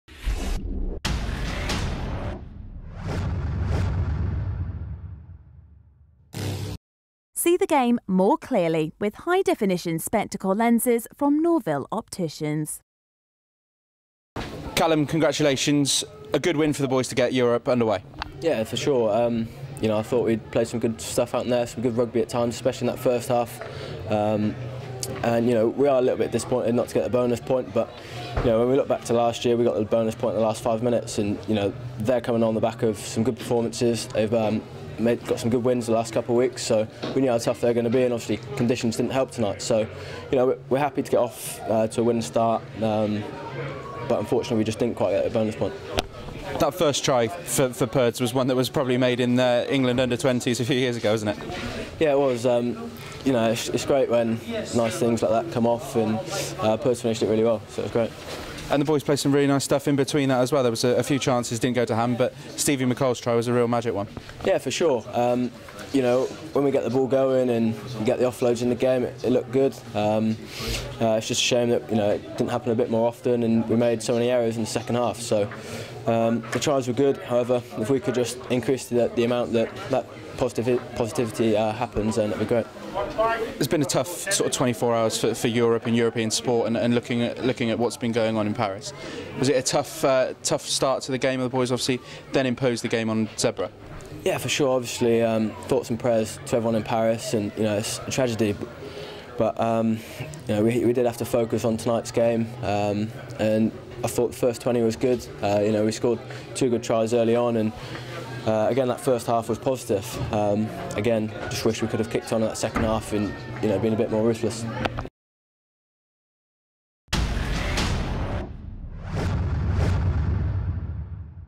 Braley happy with first run start of the season in win over Zebre
Speaking to GRTV after the 23-10 win over Zebre, scrum half Callum Braley spoke of his happiness at getting the new Challenge Cup campaign off to a winning note.